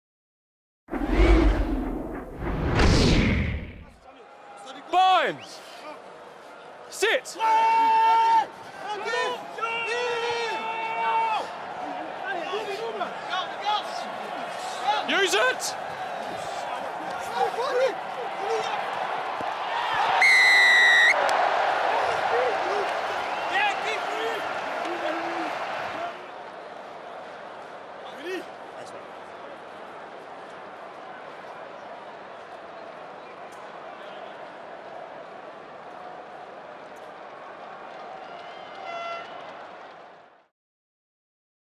Picamoles powers over after attacking 5m scrum! | RBS 6 Nations
Strong French no.8 Louis Picamoles went over for a try after a well worked French scrum.

Italy v France | 11 March 2017 | Stadio Olimpico

Italy: 15 Edoardo Padovani, 14 Angelo Esposito, 13 Michele Campagnaro, 12 Luke McLean, 11 Giovanbattista Venditti, 10 Carla Canna, 9 Edoardo Gori, 8 Sergio Parisse (c), 7 Simone Favaro, 6 Abraham Steyn, 5 Dries van Schalkwyk, 4 Marco Fuser, 3 Lorenzo Cittadini, 2 Leonardo Ghiraldini, 1 Andrea Lovotti
Replacements: 16 Tommaso D’Apice, 17 Sami Panico, 18 Dario Chistolini, 19 George Biagi, 20 Maxime Mbanda, 21 Giorgio Bronzini, 22 Tommaso Benvenuti, 23 Luca Sperandio

France: 15 Brice Dulin, 14 Noa Nakaitaci, 13 Rémi Lamerat, 12 Gaël Fickou, 11 Virimi Vakatawa, 10 Camille Lopez, 9 Baptiste Serin, 8 Louis Picamoles, 7 Kévin Gourdon, 6 Fabien Sanconnie, 5 Yoann Maestri, 4 Julien Le Devedec, 3 Rabah Slimani, 2 Guilhem Guirado (c), 1 Cyril Baille
Replacements: 16 Christopher Tolofua, 17 Uini Atonio, 18 Eddy Ben Arous, 19 Paul Jedrasiak, 20 Bernard Le Roux, 21 Antoine Dupont, 22 Francois Trinh-Duc, 23 Yoann Huget